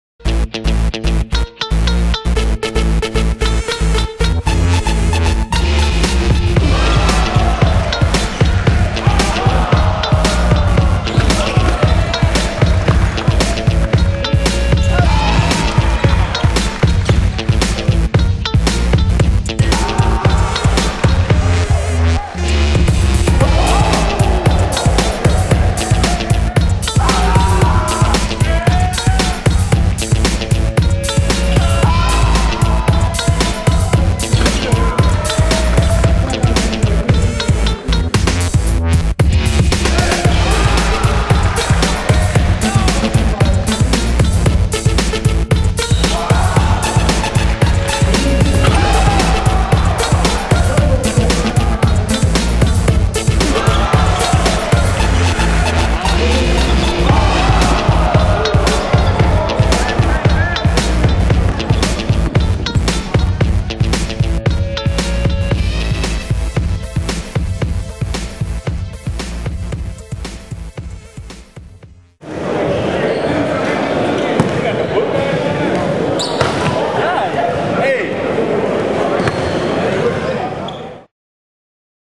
6'3 Deuce Bello Gets Dunks ALL Day; Crazy Athletic Wing
Here is D-One Sports own, Deuce Bello doing his thing at King City, Pangos, and the Reebok Summer Championships In Vegas. Bello gets a crazy amount of dunks for only being 6'3. Deuce will be a Junior in high school next year (Class Of 2011).